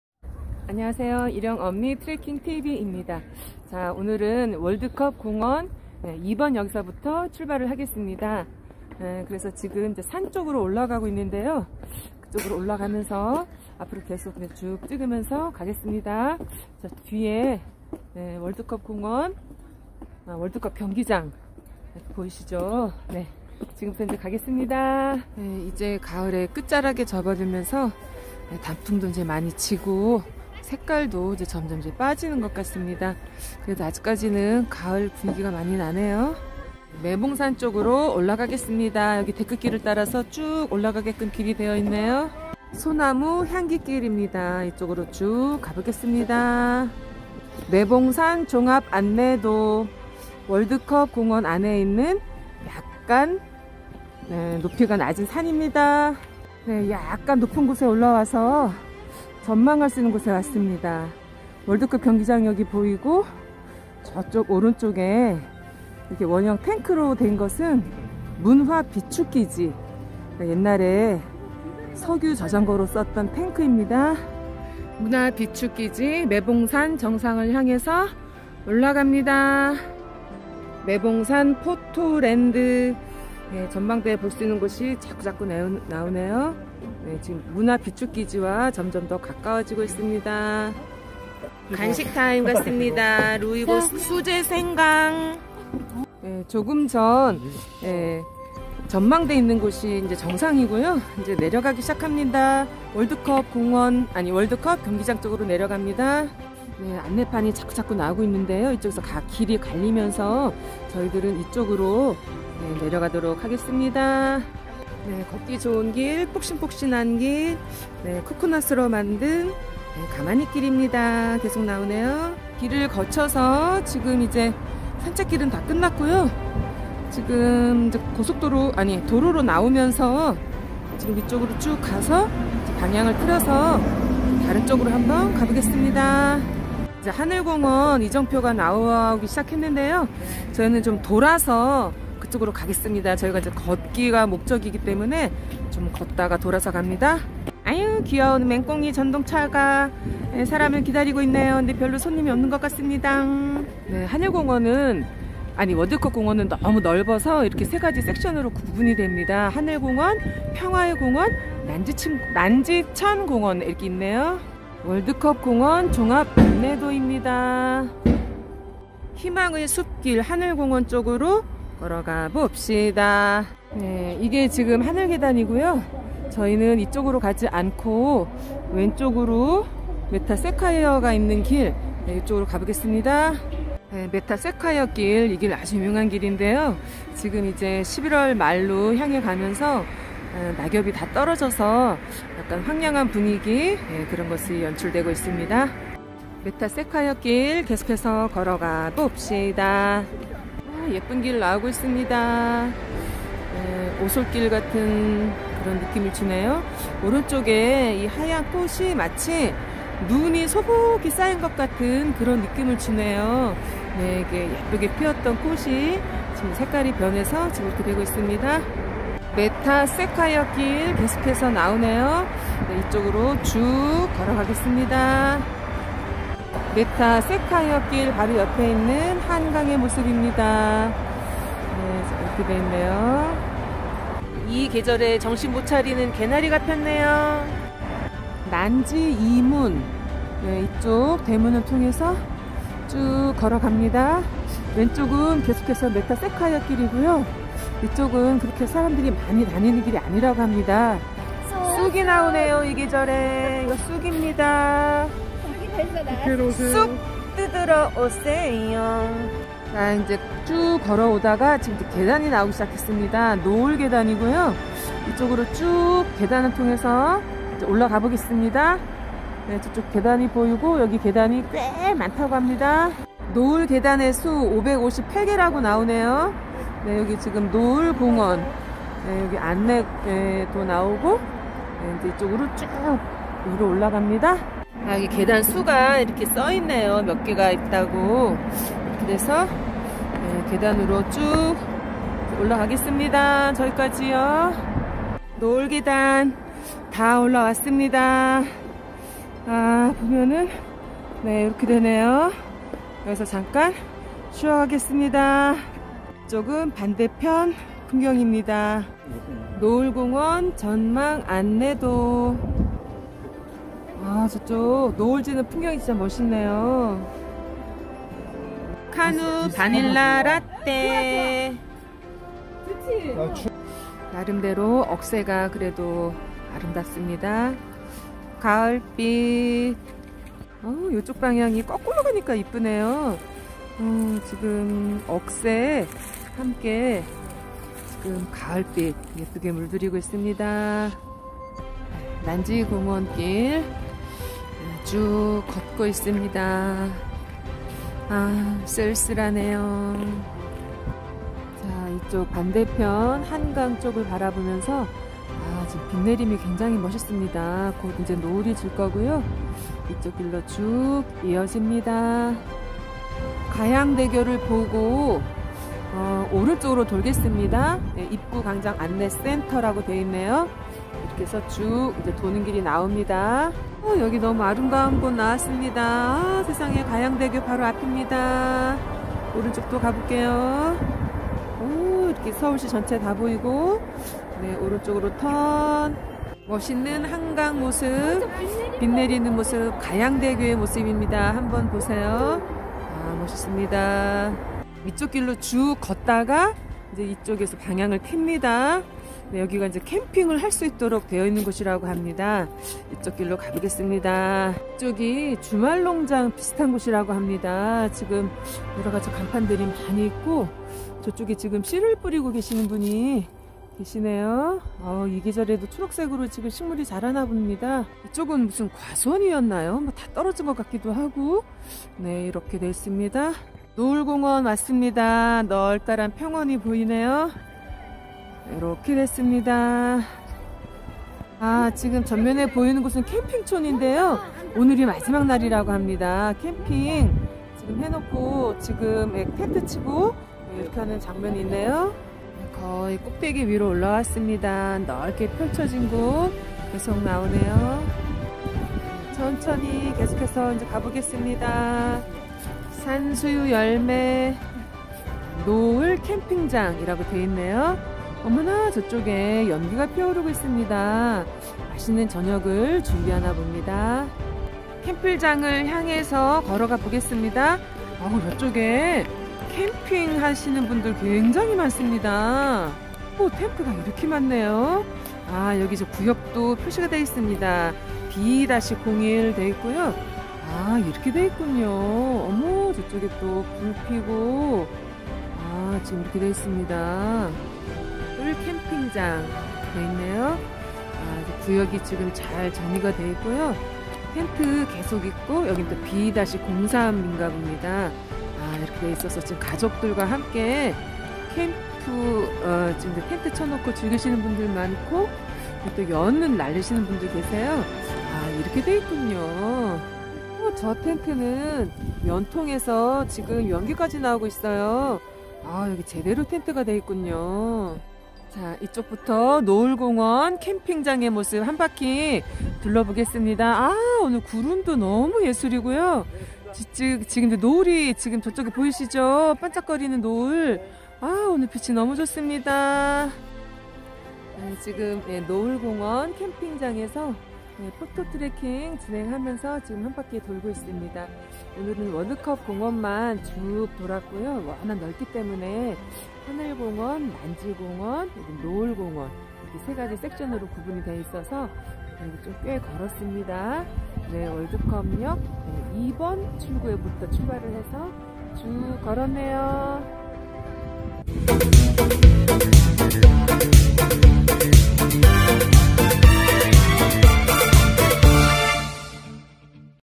노을이 아름다운 월드컵공원의 노을캠핑장
20.11.29 월드컵공원을 크게 돌고 노을이 아름답고 캠핑을 할 수 있는 노을캠핑장을 감